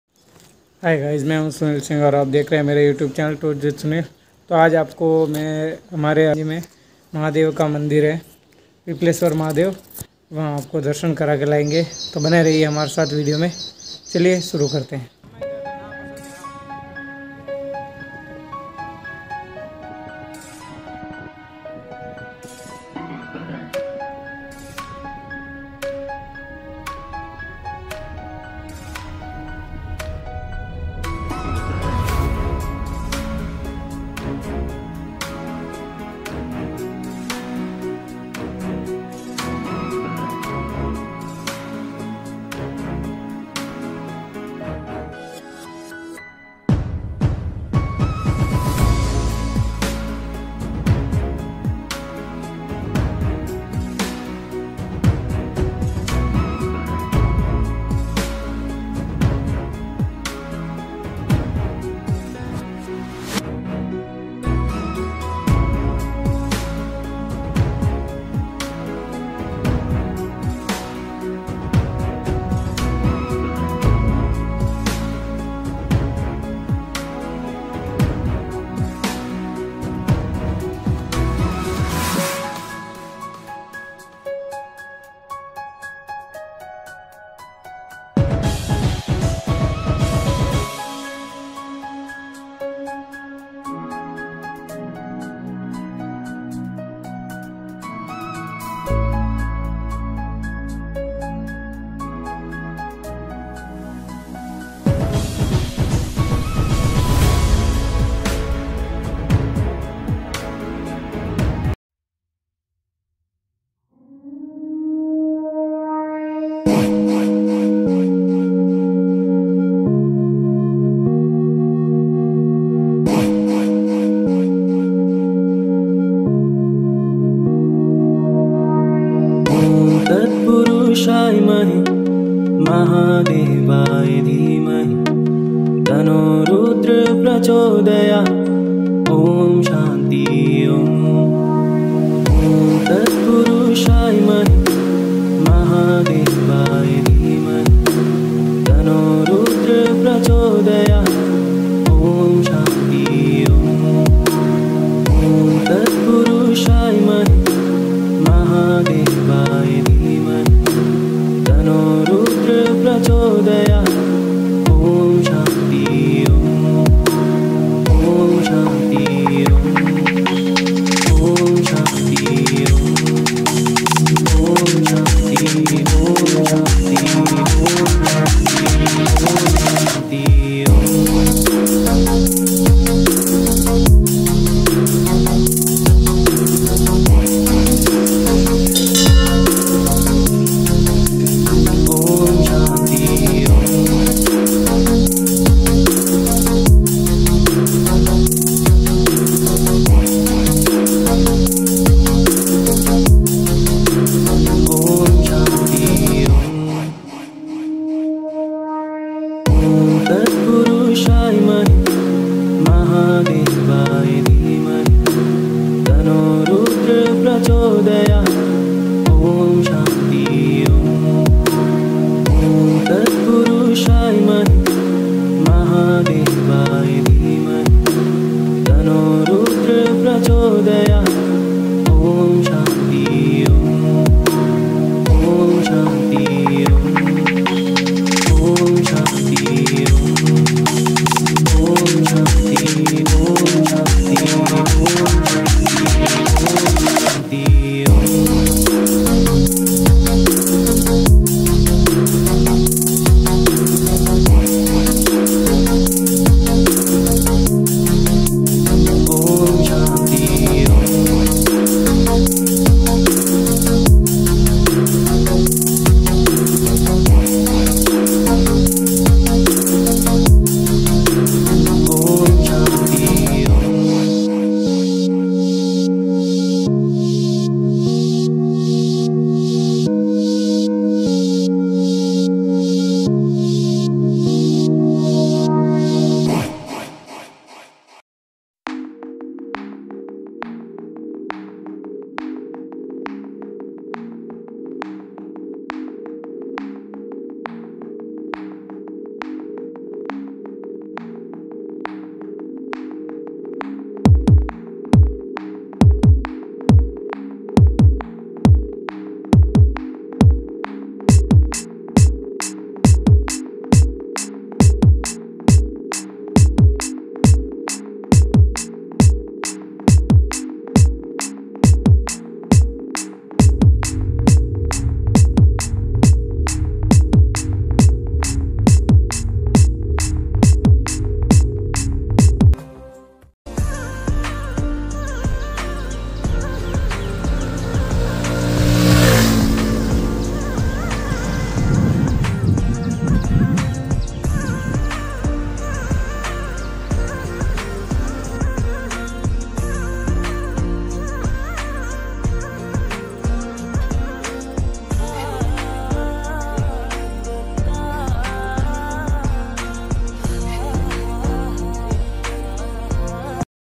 Pipleshwar Mahadev Temple Kalab Kalan (महादेव) 2021 Lord shiva
Yeh Video Pipleshwar Mahade Temple Se Shuoot Kya Gaya Hai. Yeh Temple Raipur Se 20 km Door He.Yeh Aravali Forest Me Aata He .

instagram id ;- mr_banna773 / mr_banna121